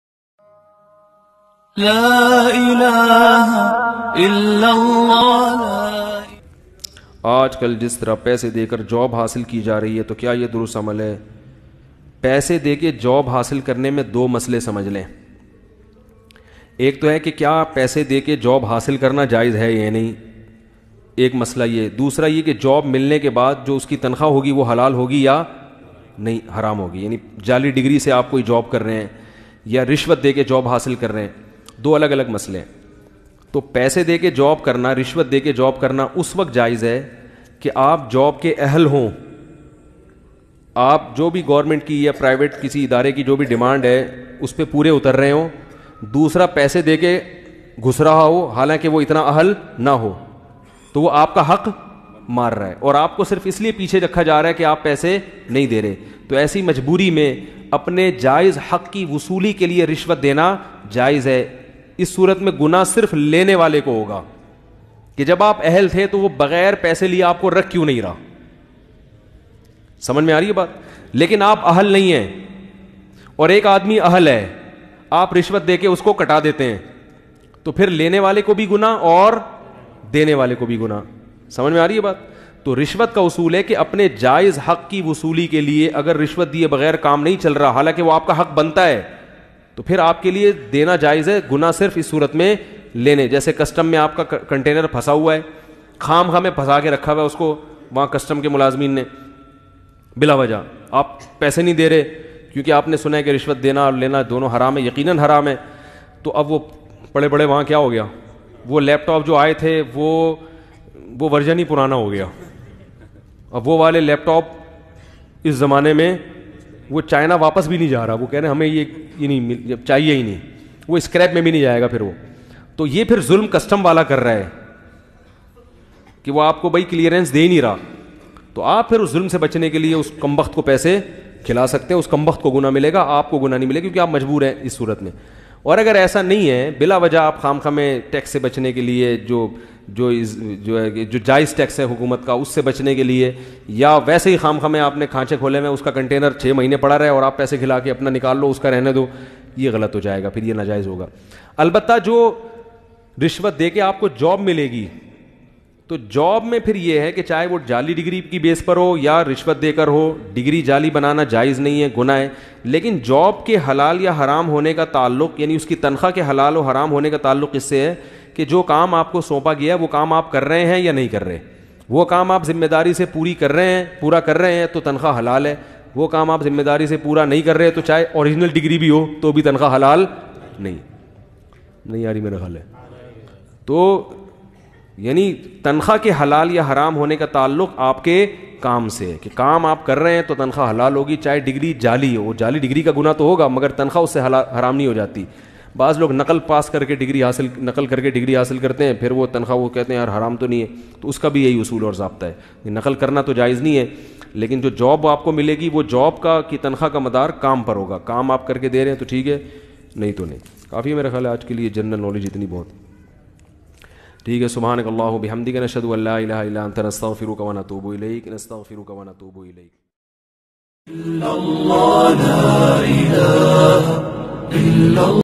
Kiya Apne Haq Ke Hasool Ke Liye Rishwat Dena Jaiz Hai? Mufti Tariq Masood || Islamic Rehber
مفتی طارق مسعودصاحب
کیا اپنے حق کے حصول کے لیے رشوت دینا جائز ہے؟

ایسی اور دینی اور معلوماتی ویڈیوز کیلئے ہمارے یوٹیوب چینل کو سبسکرائب ضرور کریں۔۔۔